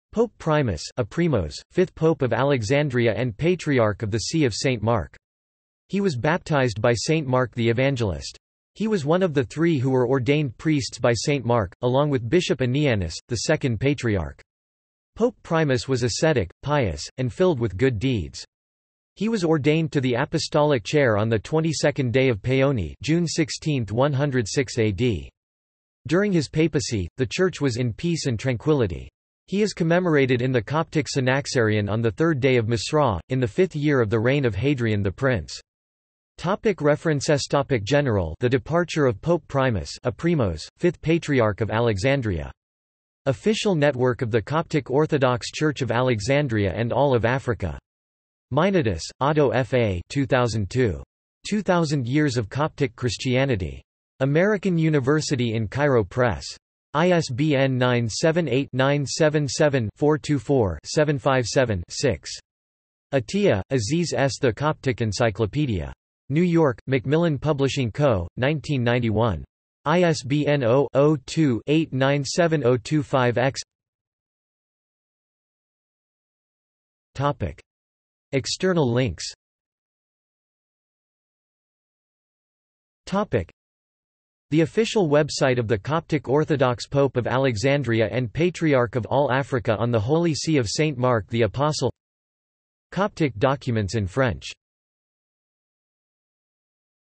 Pope Primus of Alexandria | Wikipedia audio article
This is an audio version of the Wikipedia Article:
Pope Primus of Alexandria

SUMMARY
Pope Primus (Aprimos),  fifth Pope of Alexandria & Patriarch of the See of St. Mark. He was baptized by St. Mark the Evangelist. He was one of the three who were ordained priests by St. Mark, along with Bishop Anianus, the Second Patriarch. Pope Primus was ascetic, pious, and filled with good deeds. He was ordained to the apostolic chair on the 22nd day of Paoni (June 16, 106 A.D.). During his papacy, the church was in peace and tranquility. He is commemorated in the Coptic Synaxarion on the 3rd day of Mesra, in the fifth year of the reign of Hadrian the prince.